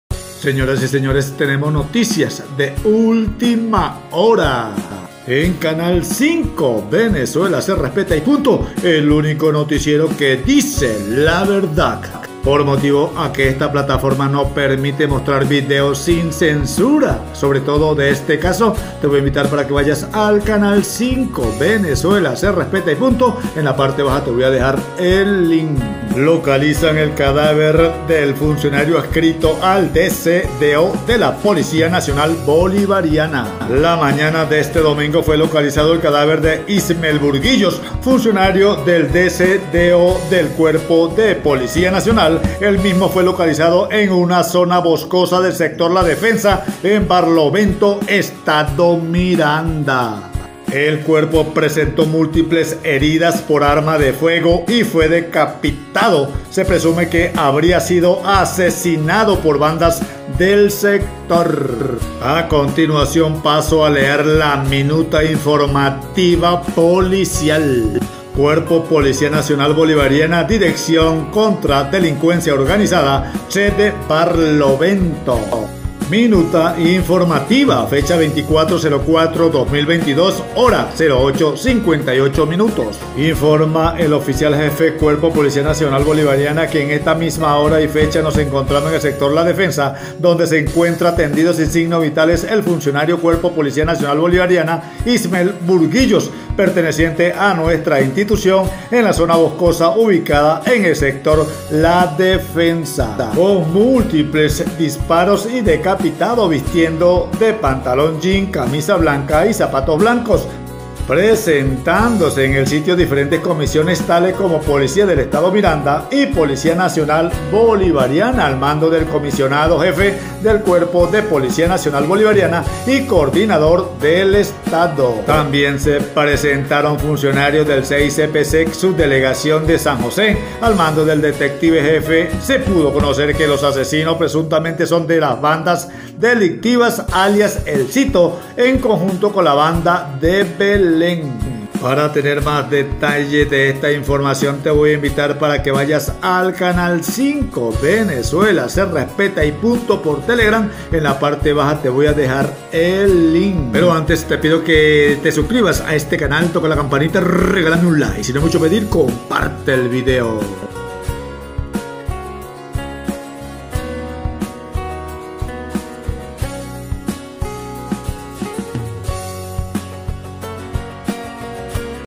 NOTICIAS Sucesos.ACABA DE SUCEDER.LE QUITARON LA VIDA DE UNA FORMA NUNCA VISTO.
le quitaron la vida al funcionario Ismael Burguillos de la policía nacional bolivariana en Miranda.